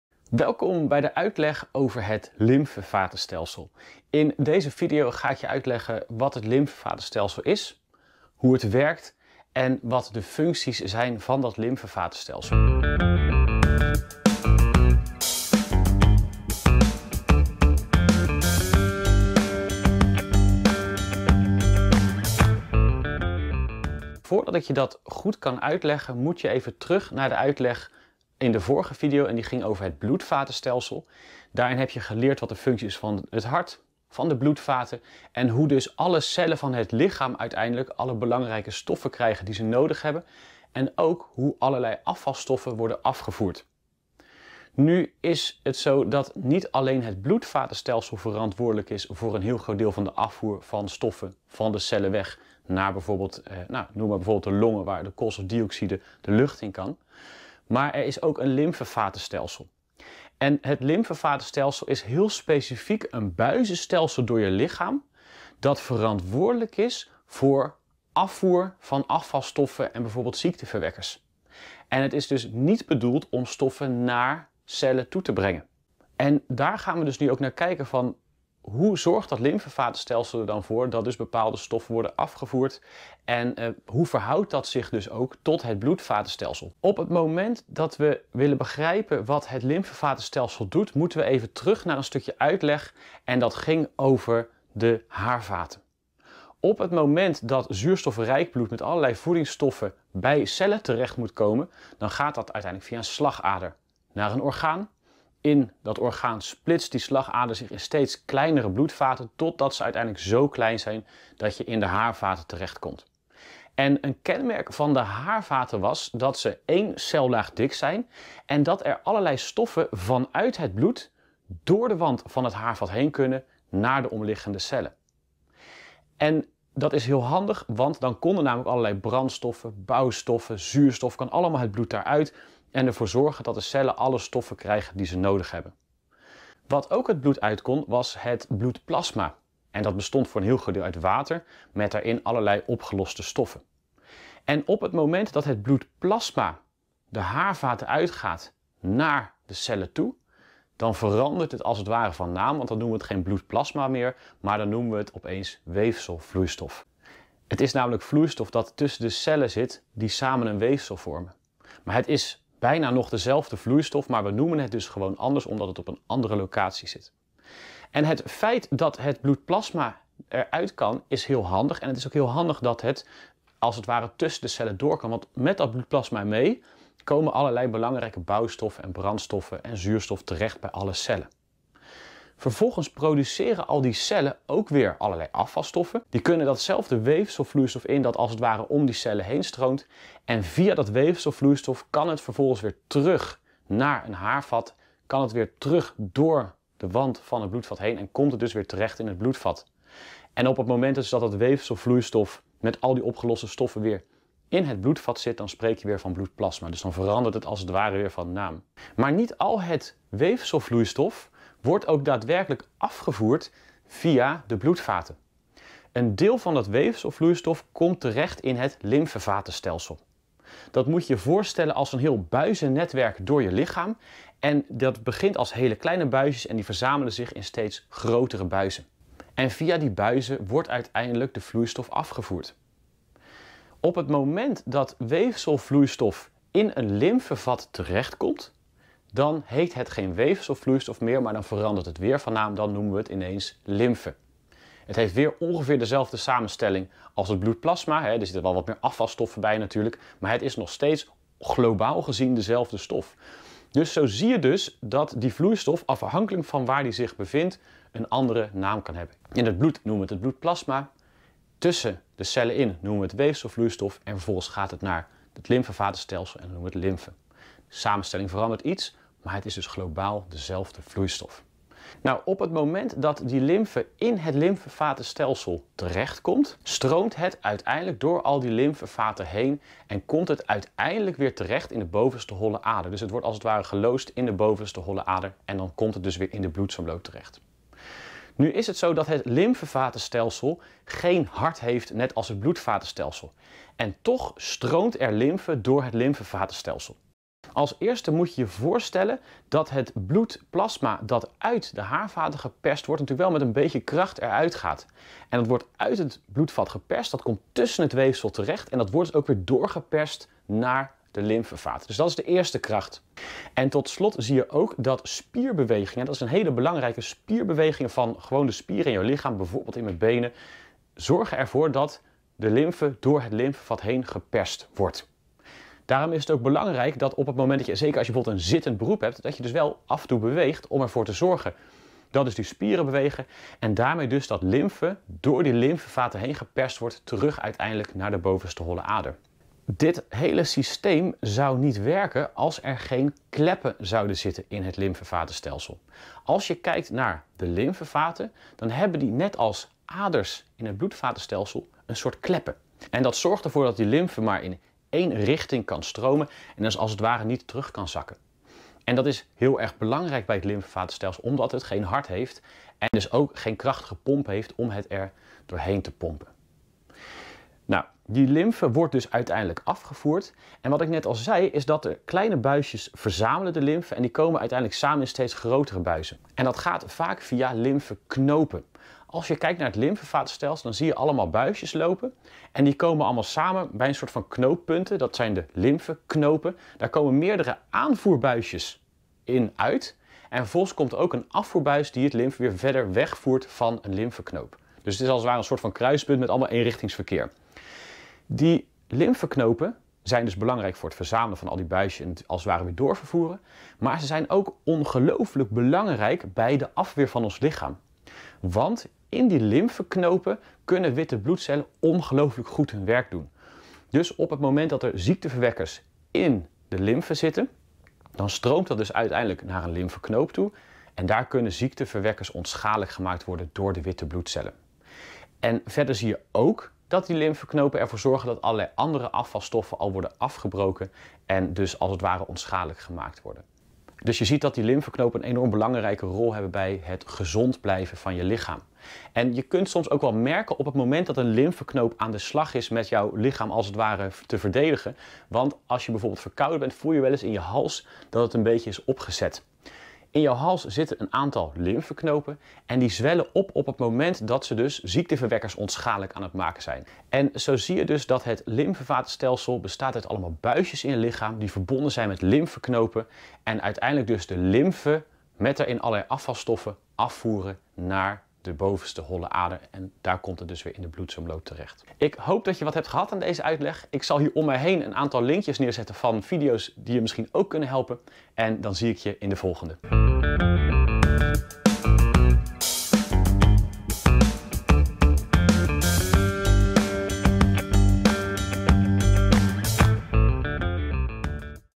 Het lymfevatenstelsel | Samenvatting
In deze video leer je:
-Uit welke onderdelen het lymfevatenstelsel bestaat.
- Welke functie het lymfevatenstelsel heeft.